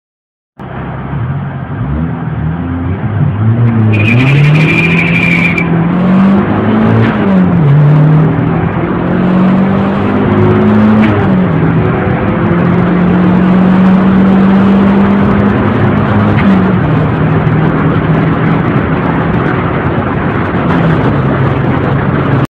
4E-FTE EP91 (in EE90 Body) Test 3 Gear 0-120Km/h
just a little test of my 4E-FTE

เทสขำๆ 3 เกียร์คับ